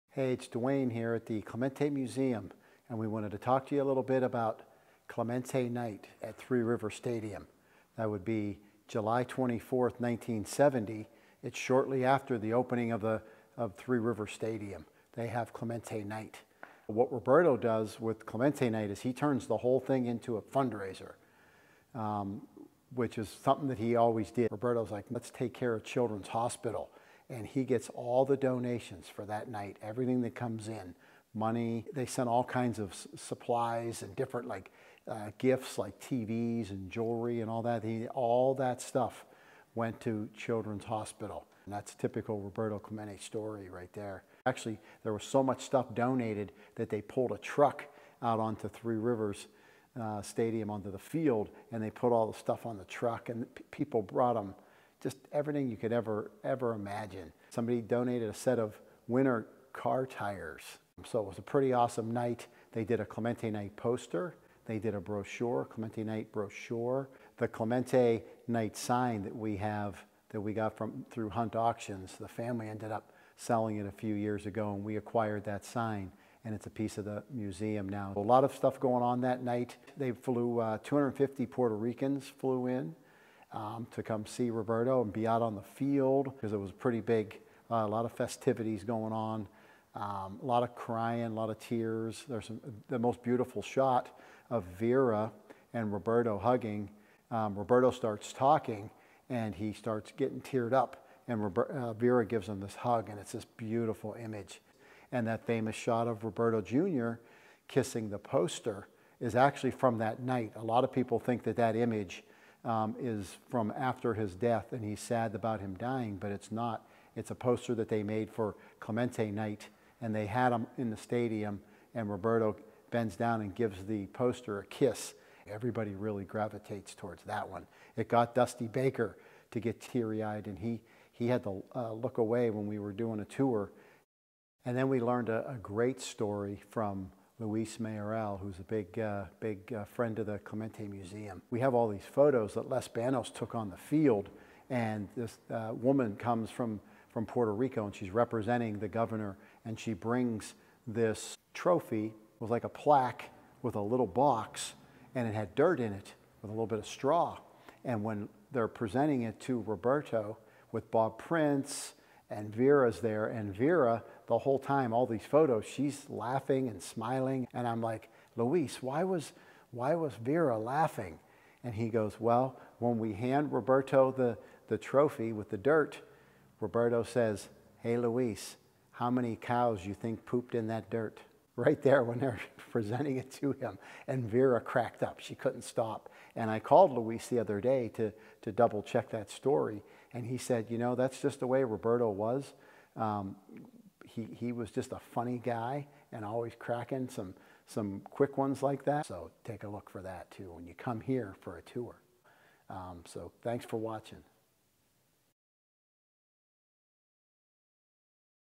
Pittsburgh Pirates Roberto Clemente Night in 1970
Learn about Clemente Night in 1970! Our curator, Duane Rieder, talks about what happened on Clemente Night in July 1970. The Pittsburgh Pirates held this night to honor Roberto Clemente and celebrate with his fans and family.